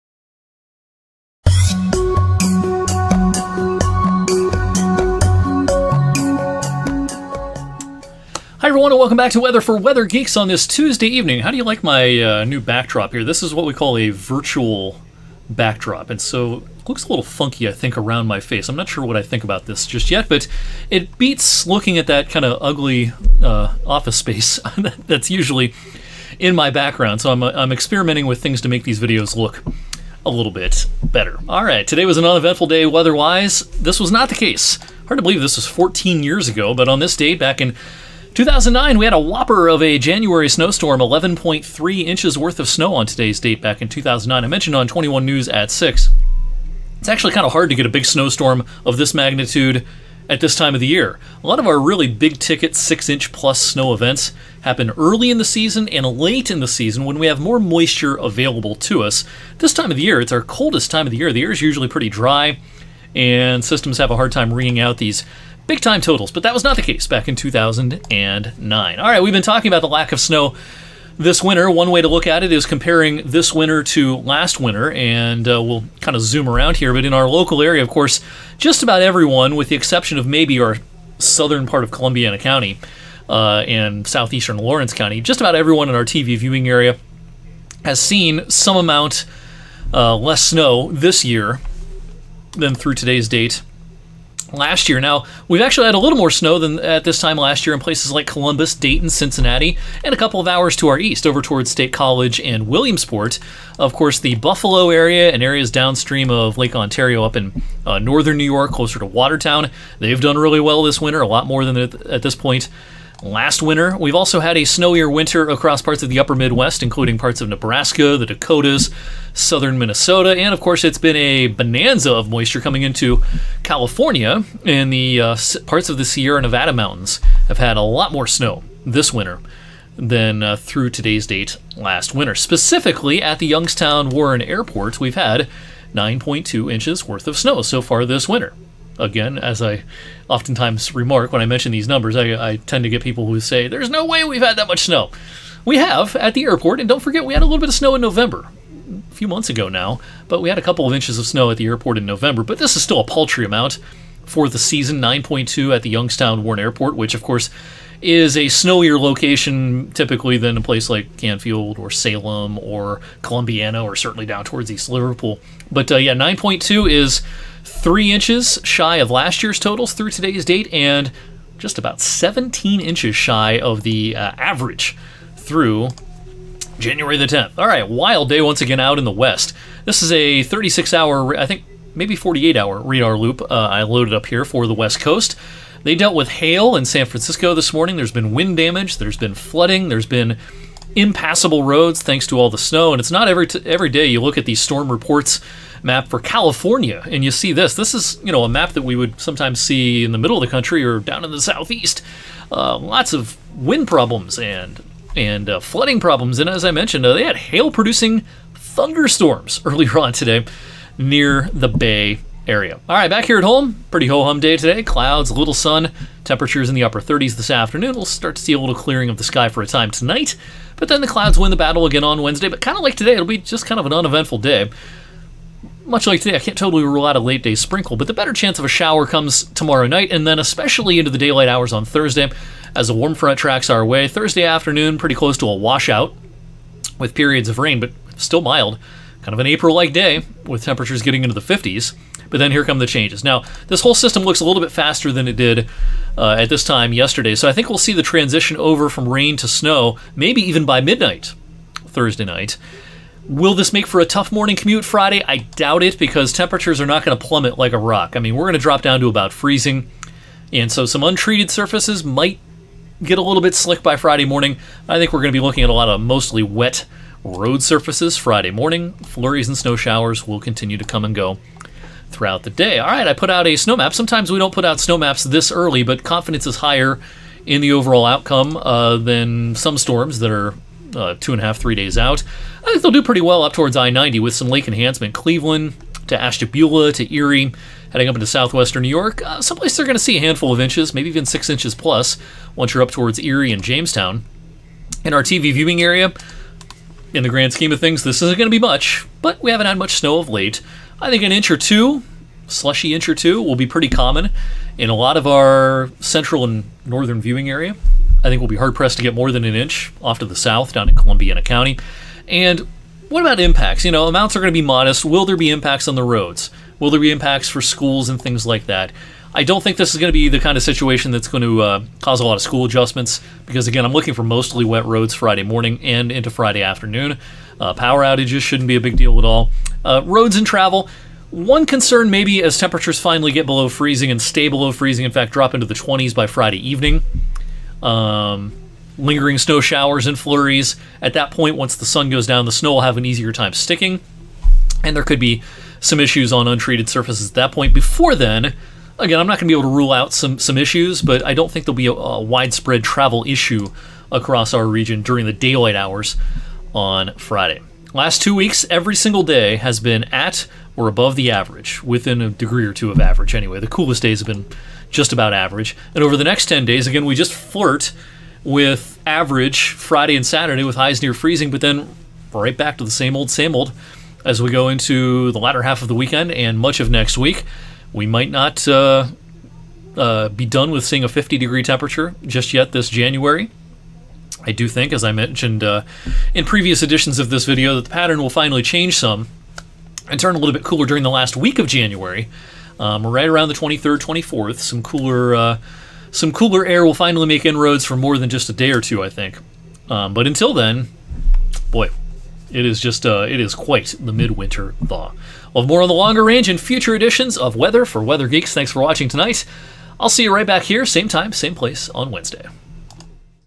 Spring Thursday, Winter Friday; "Weather For Weather Geeks" 1/10/23
00:00 Intro
00:32 2009 Snowstorm Anniversary
01:14 More Lack-Of-Snow Chat
02:58 California Deluge
03:44 Rain To Snow Late This Week
05:58 Balmy Long-Range Forecast